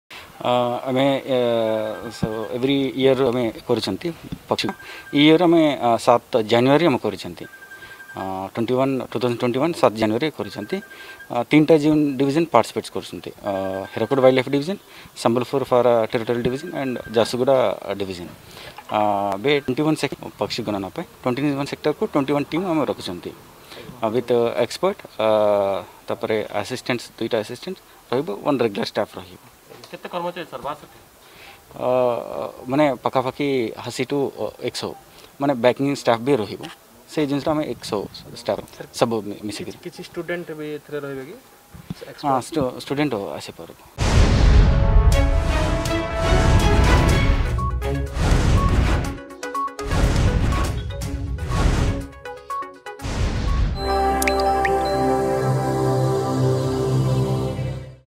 ଆସନ୍ତା ୭ ରୁ ପକ୍ଷୀ ଗଣନା
ଆସନ୍ତା ଜାନୁୟାରୀ 7 ତାରିଖରେ ହୀରାକୁଦ ଜଳଭଣ୍ଡାରରେ ହେବ ପକ୍ଷୀ ଗଣନା । 3 ଫରେଷ୍ଟ ଡିଭିଜନ ମିଶି କରିବେ ପକ୍ଷୀ ଗଣନା.